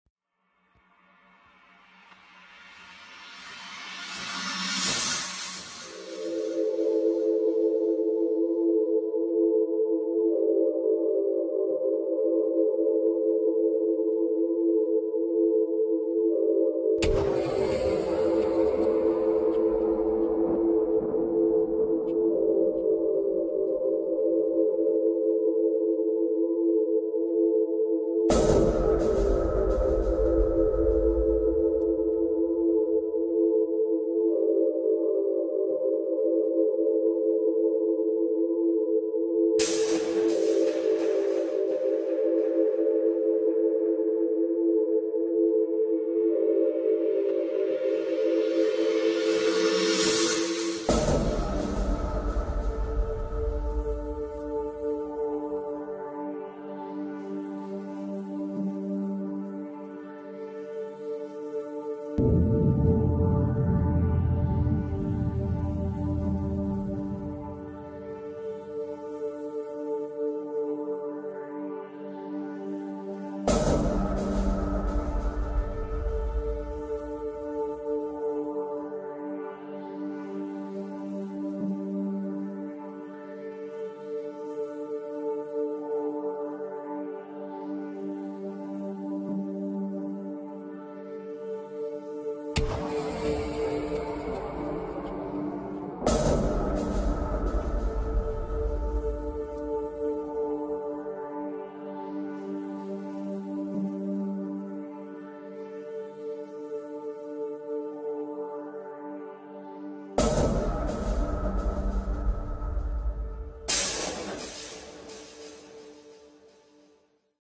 Ruinen - Schrottgebäude aus DDR Zeiten
Ruins, junk buildings from GDR times. Discovered by Google Earth. Ruins remain.
Zufällig über Google Earth entdeckt. Ruinen sind geblieben.